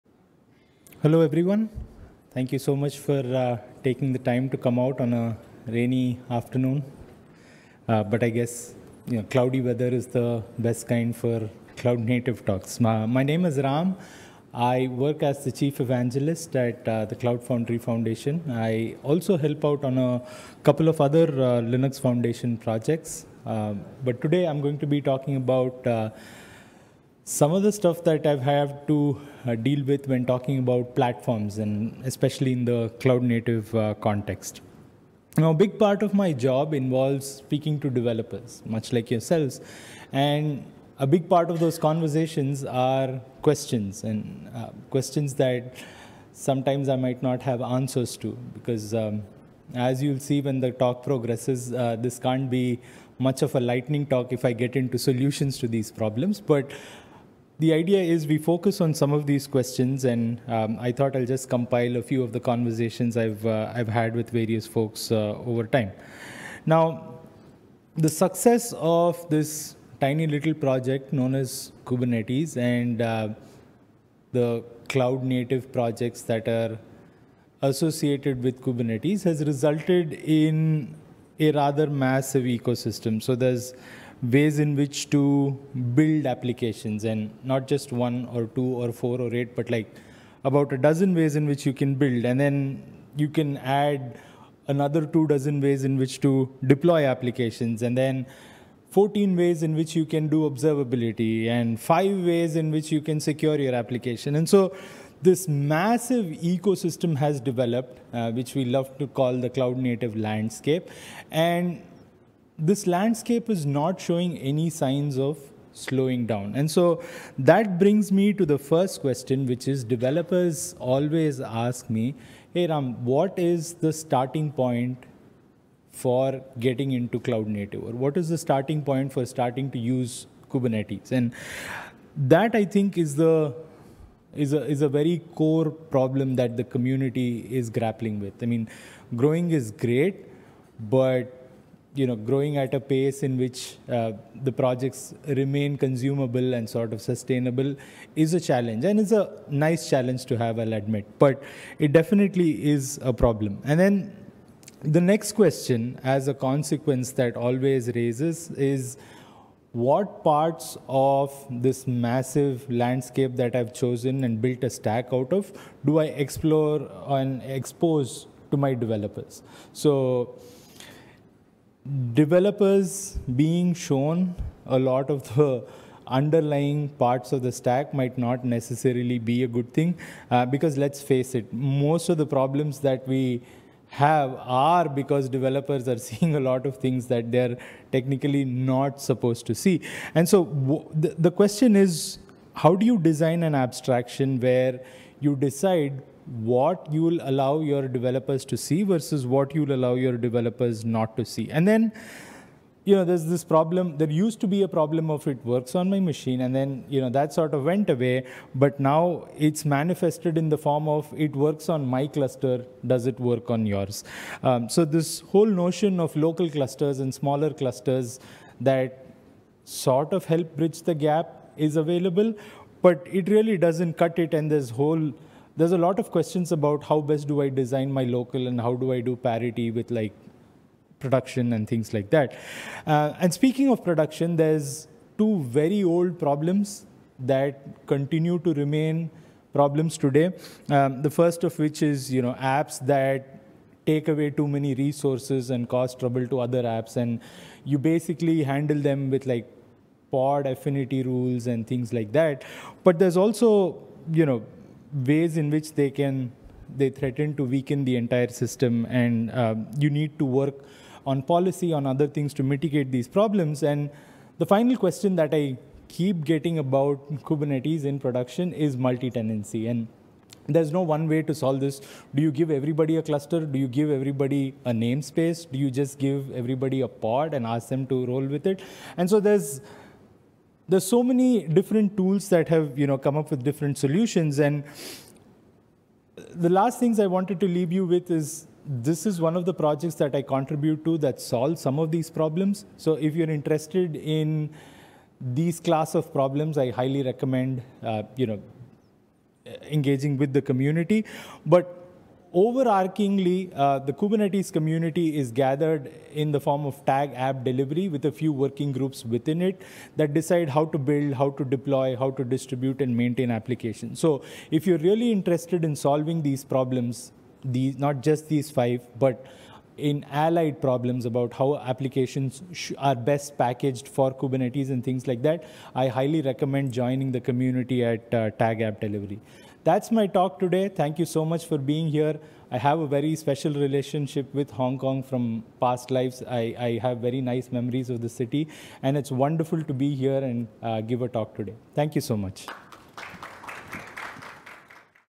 Lightning Talk: Kubernetes Raises Questions. Can a PaaS Answer Them? - Ram Iyengar
Lightning Talk: Kubernetes Raises Questions. Can a PaaS Answer Them? | ⚡ 闪电演讲: Kubernetes引发了问题。 PaaS能解答吗？ - Ram Iyengar, Cloud Foundry Foundation

The enormous success of the CNCF Landscape has produced an overwhelming number of options in the space, where organizations struggle to establish their platforms quickly. This talk will help guide the community through the thought process of building these platforms, explore some examples of what a healthy source-driven platform ecosystem looks like, and showcase the power that a good cloud native platform will deliver to an organization. Though there are variations of platforms (i.e data, application, machine learning, etc) many start to have the same problems. These include artifact management, secrets management, TLS certificates, cloud permissions, and the list goes on. Providing turnkey solutions for platforms that can be ready in minutes adds much velocity to engineering teams across organizations that adopt the platform engineering model.

CNCF景观的巨大成功在该领域产生了大量的选择，组织往往难以快速建立自己的平台。本次演讲将帮助指导社区通过构建这些平台的思考过程，探讨健康的源驱动平台生态系统的一些示例，并展示一个优秀的云原生平台将为组织带来的力量。 尽管平台有各种变化（如数据、应用程序、机器学习等），许多开始出现相同的问题。这些问题包括工件管理、密钥管理、TLS证书、云权限等等。为平台提供即插即用的解决方案，可以在几分钟内准备就绪，为采用平台工程模型的组织的工程团队带来更大的速度。